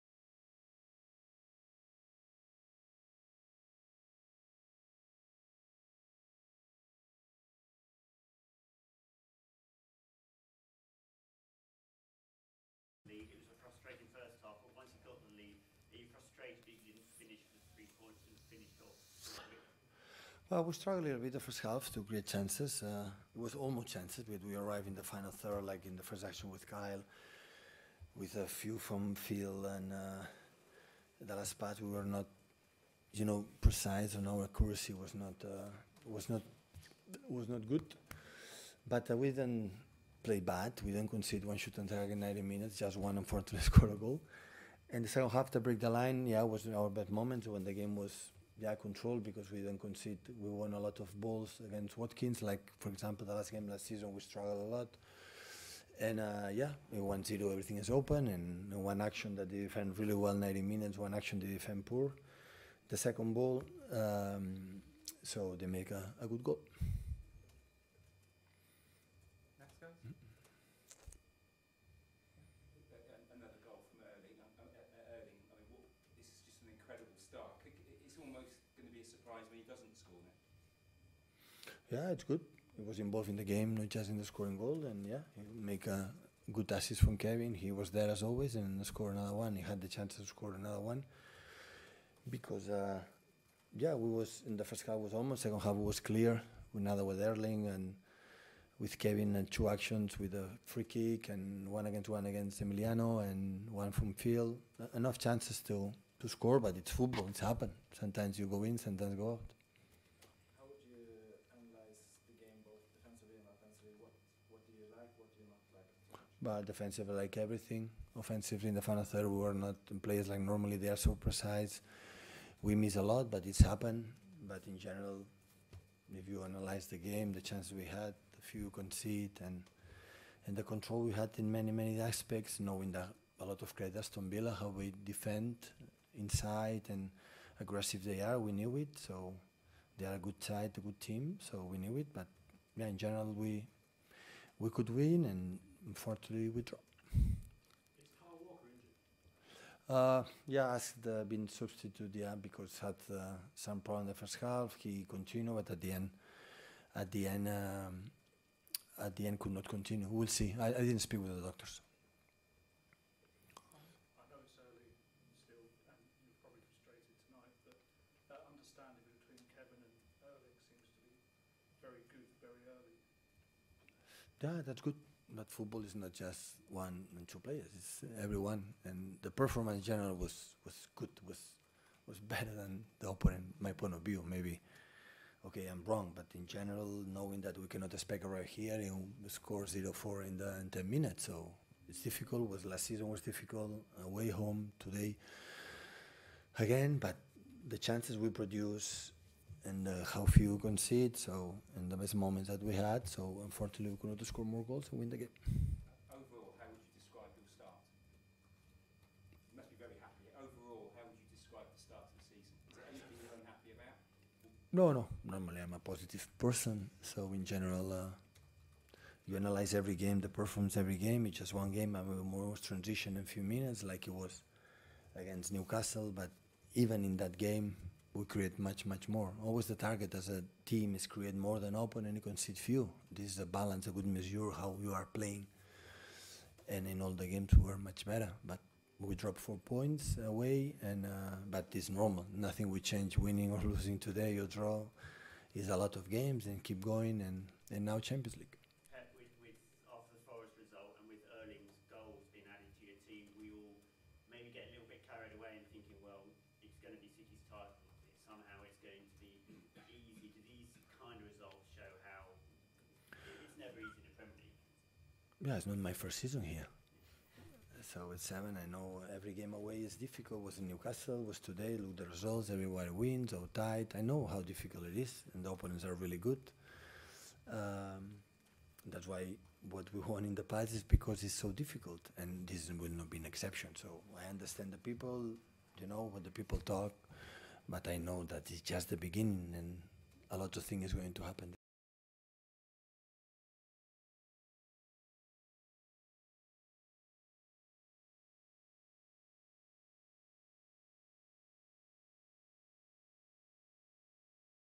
Aston villa 1 vs Man City 1 | Pep Guardiola | Post-Match Press Conference
Pep Guardiola Speaks after the game vs Aston Villa that ended in a draw 1-1.